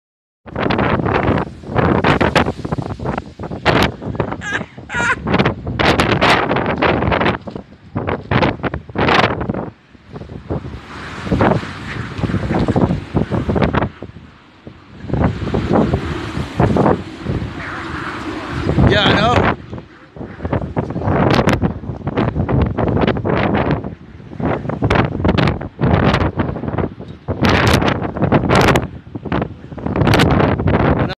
Blown Off Course
55 Mph winds, Huge wave's hit pier... Millport isle of Cumbrae.